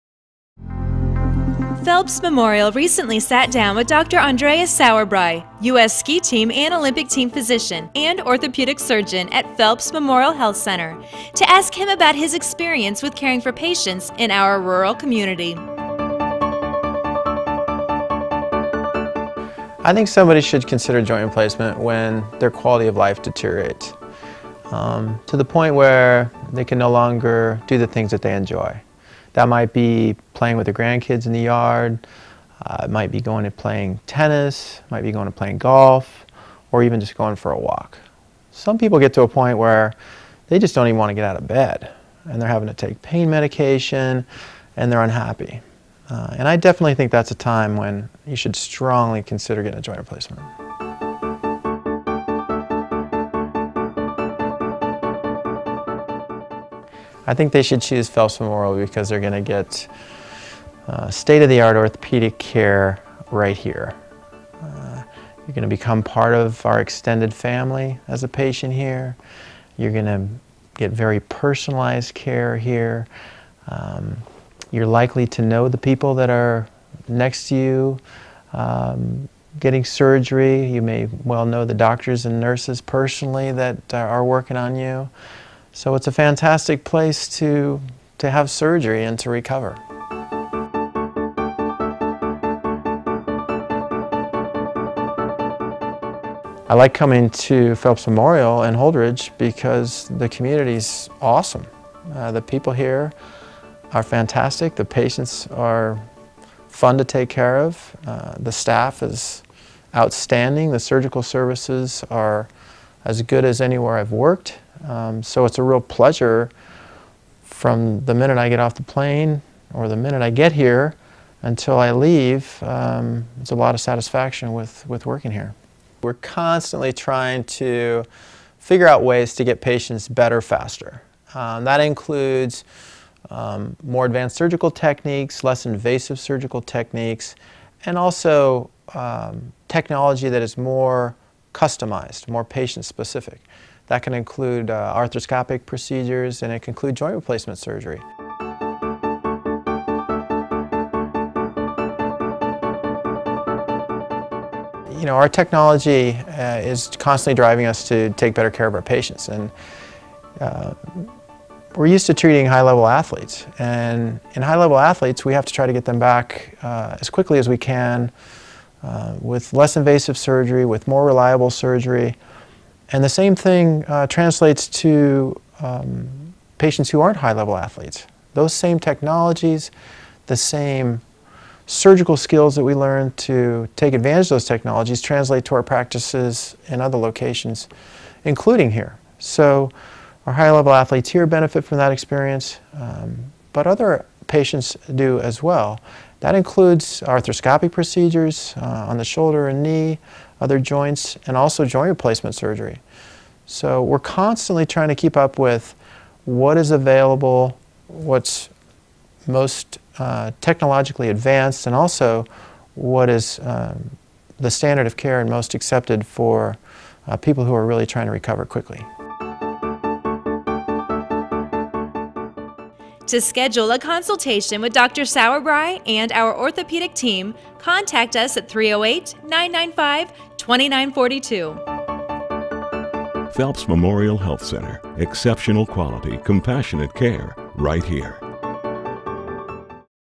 Dr. Andreas Sauerbrey, Orthopedic Surgeon
Dr. Andreas Sauerbrey, Orthopedic Surgeon, is trained to manage all aspects of orthopedic care including joint replacement, sports and trauma. He has extensive experience and interest in Platelet Rich Plasma therapy.